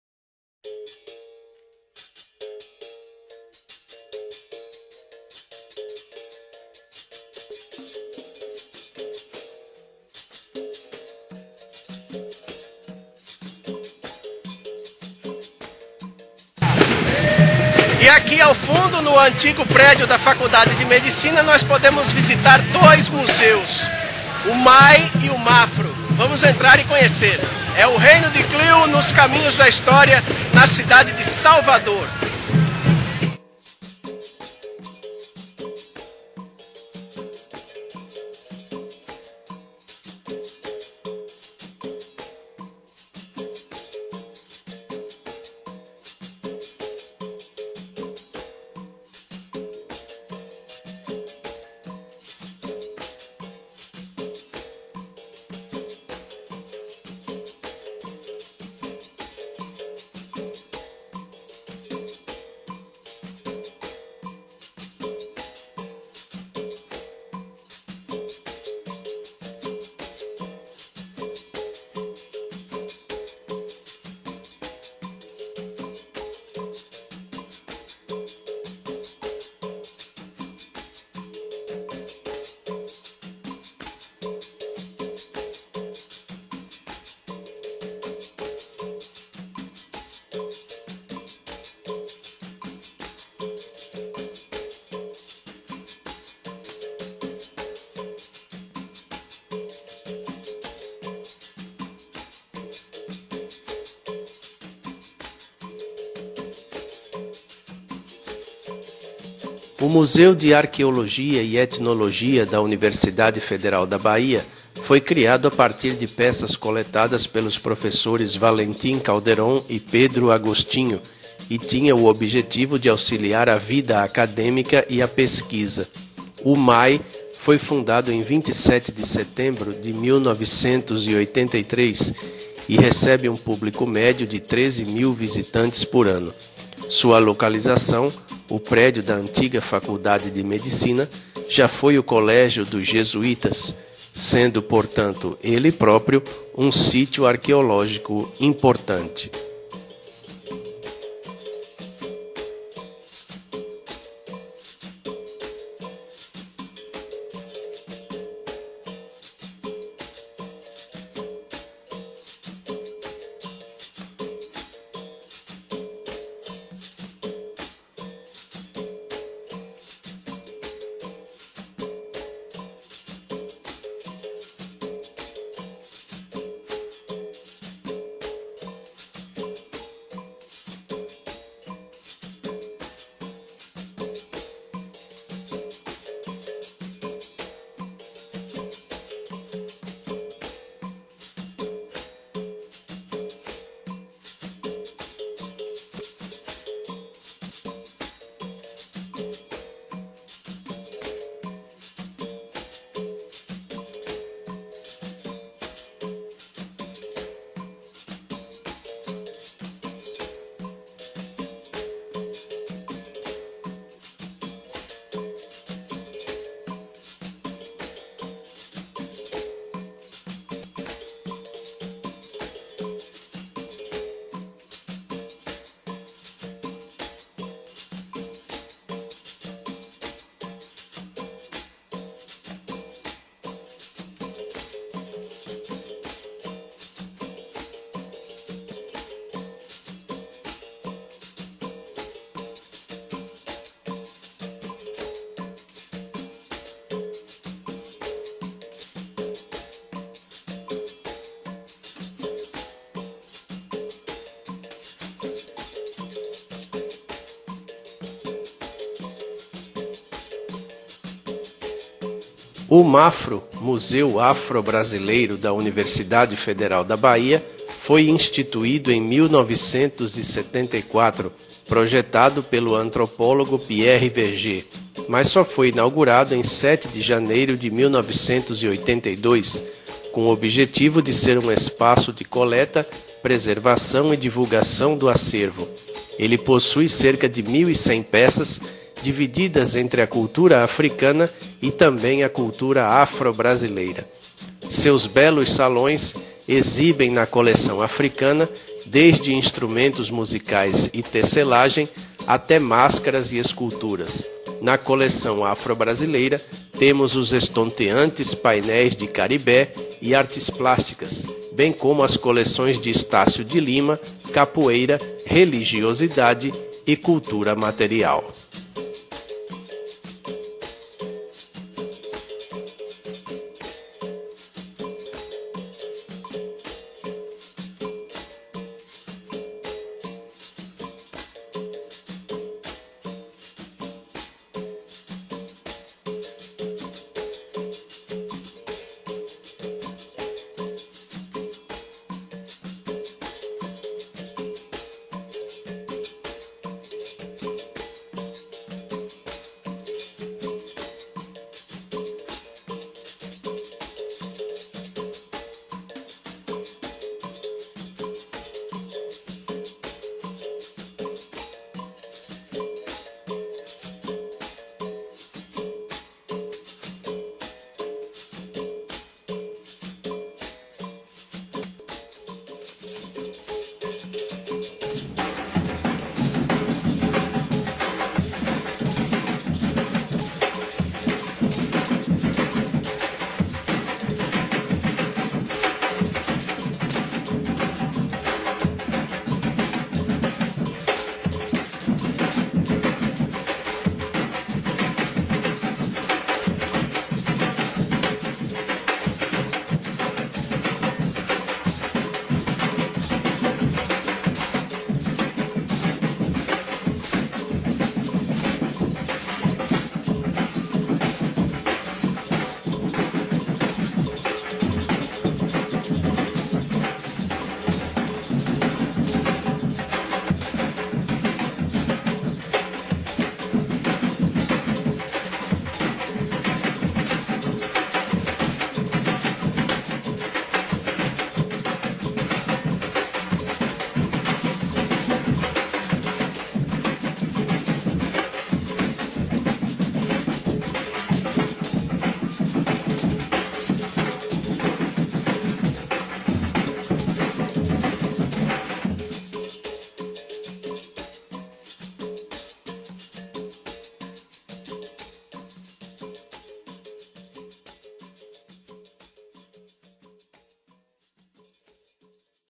Museu Afro Brasileiro e Museu de Arqueologia e Etnologia da Bahia
Sétimo episódio da série Caminhos da História em Salvador: Pelourinho. Visita ao MAFRO - Museu Afro Brasileiro e ao M.A.E. - Museu de Arqueologia e Etnologia da Bahia. As fotos estão disponíveis nos links abaixo:

M.A.E.